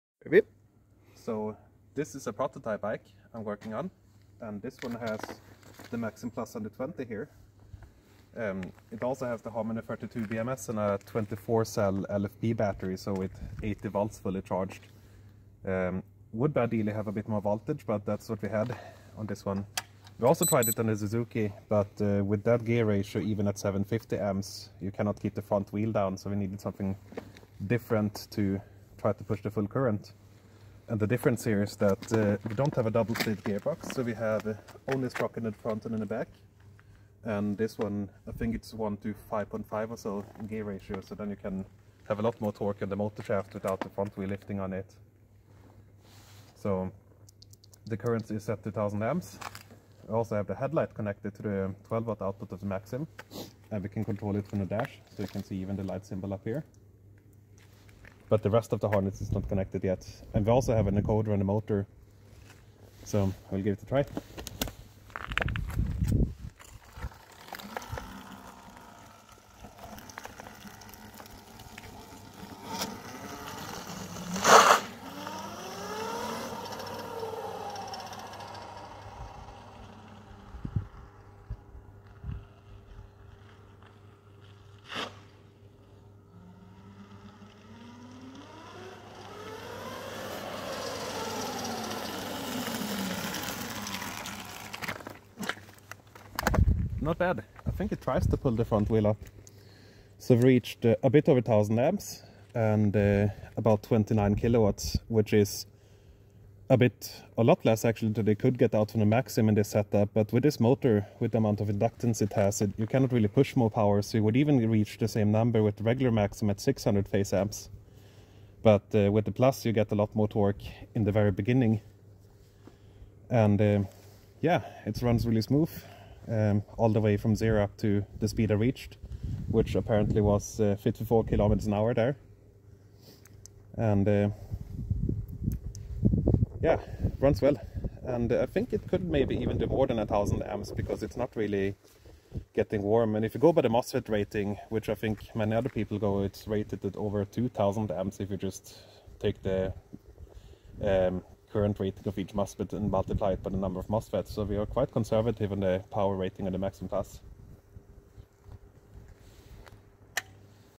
Maxim+ 120 1000A Phase Current Test
The first production batch of the Maxim+ 120 and Maxim+ 150 have arrived and here I'm testing a Maxim+ 120 with 1000A phase current on a bike that I'm working on.

It runs perfectly smooth at 1000A phase current and I'm really happy with how it performs. We have been working on the Maxim+ for more than a year now and before the production batch we did 4 rounds of prototypes, so I'm confident that it is a good and reliable ESC.

* Small correction: I mentioned in the video that the MOSFET rating is over 2000A, but that was a mistake of me. The combined MOSFET rating is
1700A at 25C
1200A at 100C